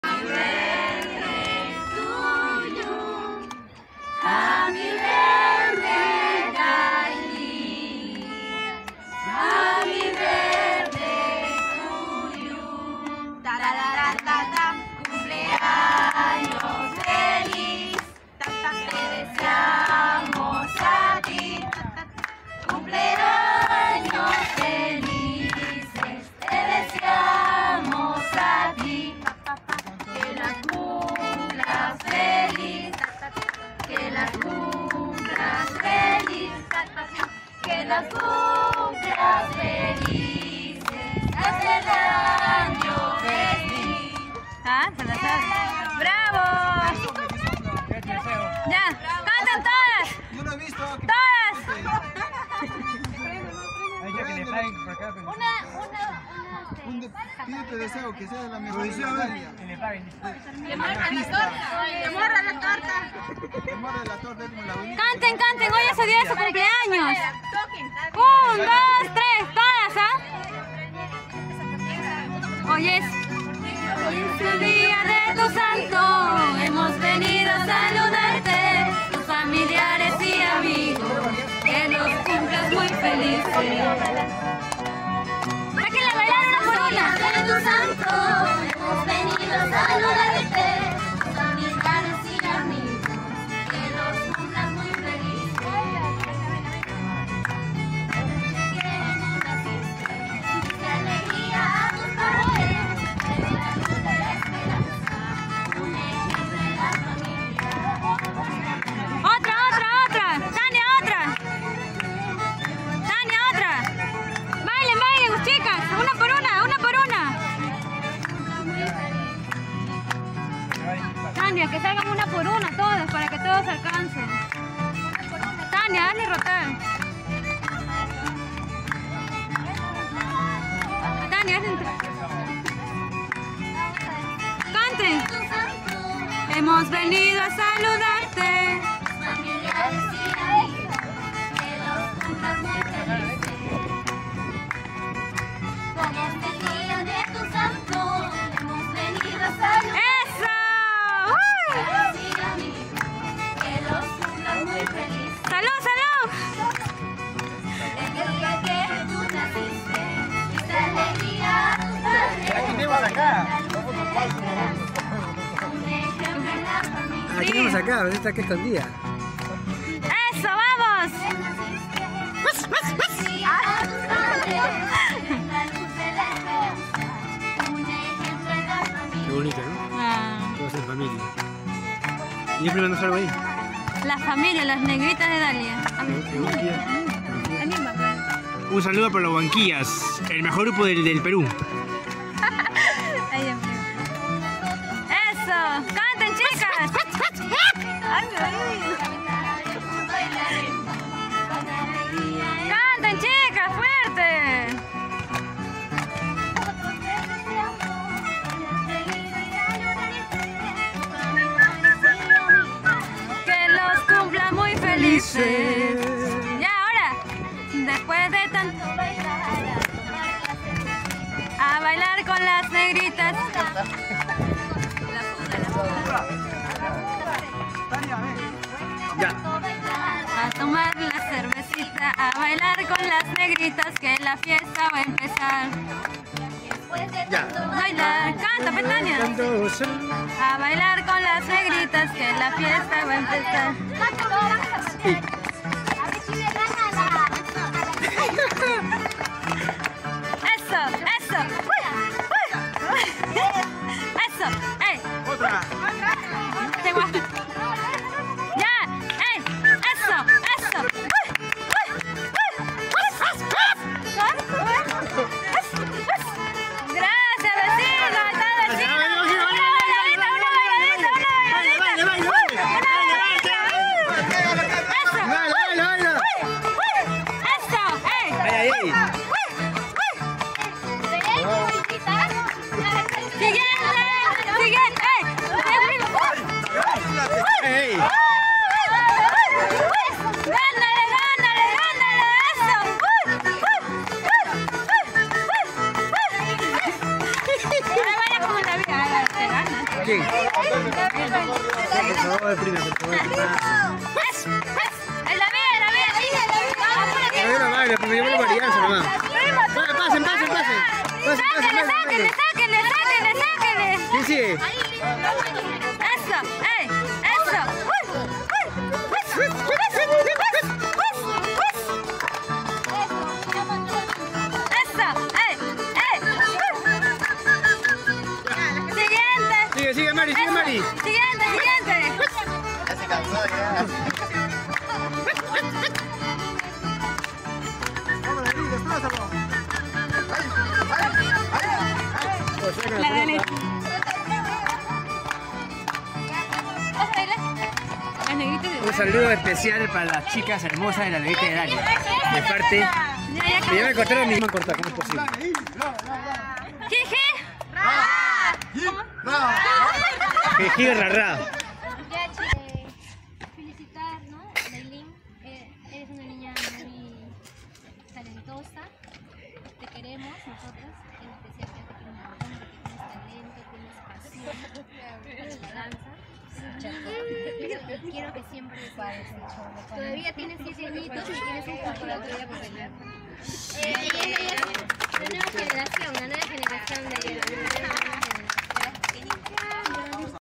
Feliz cumpleaños DAYLIN.
Feliz cumpleaños DAYLIN ( la integrante mas pequeña del grupo de Las Negritas de Dalia.) un pequeño reunión en el cumpleaños de nuestra integrante del grupo gracias por asistir familiares y amigos.